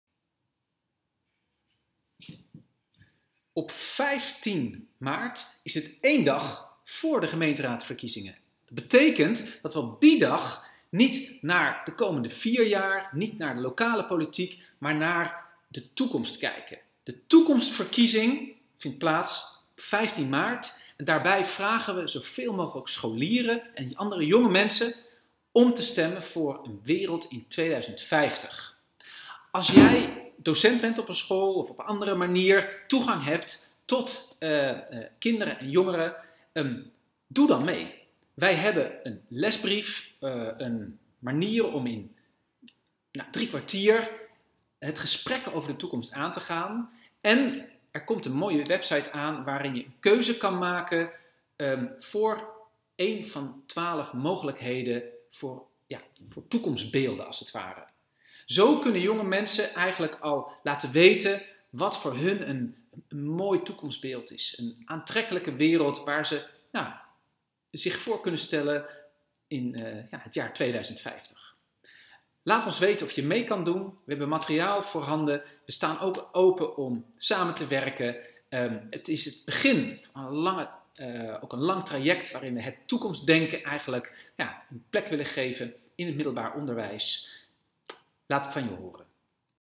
Toekomstverkiezing - korte introductie voor docenten VO
Een actie voor jongeren-perspectief op de gemeentepolitiek.

Op 15 maart 2022, organiseert een collectief van kinderen, jongeren en maatschappelijke organisaties de Toekomstverkiezing. Hierin kun je stemmen voor het Nederland van 2050. Voor docenten is er een les gemaakt van 45 minuten waarin je met de klas deel kunt nemen aan de Toekomstverkiezing.

Aan deze verkiezing doen geen politieke partijen mee, maar twaalf toekomstbeelden die elk een bepaald ideaal vertegenwoordigen. Scholieren kunnen stemmen voor een aansprekende rol in een goede wereld. De resultaten worden in elke gemeente aangeboden aan de nieuwgekozen gemeenteraad, vergezeld van een uitnodiging om jongeren structureel uit te nodigen bij beslissingen over de toekomst.

De initiatiefnemers maken zich zorgen over het gebrek aan langetermijndenken. Niet alleen in de lokale politiek, maar ook in het onderwijs en in de hele samenleving. Door het gesprek aan te gaan over de wereld van 2050, willen zij opgroeiende mensen helpen om een rol te kiezen voor zichzelf bij grote veranderingen in de wereld.
 “We zijn door de pandemie voortdurend met onszelf bezig geweest. Met problemen, maatregelen, discussies en beschuldigingen. En nu worden we gegrepen door de oorlog in Oekraïne. Natuurlijk willen we allemaal terug naar een normale wereld, maar wat is dat eigenlijk? Wij willen daarom aan heel Nederland de vraag stellen: Wat is voor jou een goede wereld?” Aldus Merlijn Twaalfhoven, componist en schrijver.

Docenten die mee willen en kunnen denken over toekomstkunde en lessen rondom het langetermijndenken kunnen zich hier aanmelden